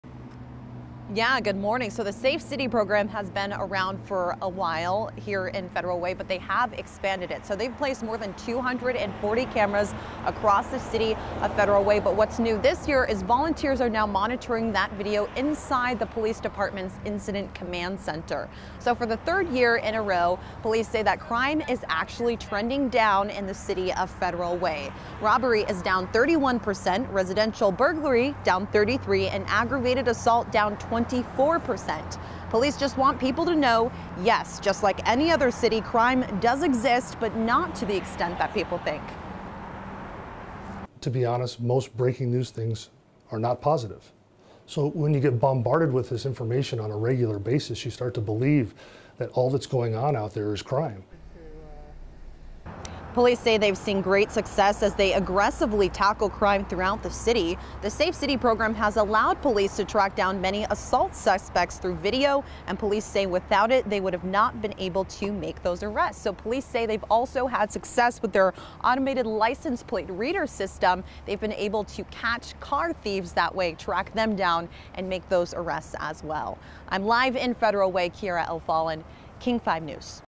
Federal Way police say crime down, despite belief that it is worse
Federal Way's Safe City program has placed more than 240 cameras throughout the city, and now volunteers monitor video inside the Incident Command Center.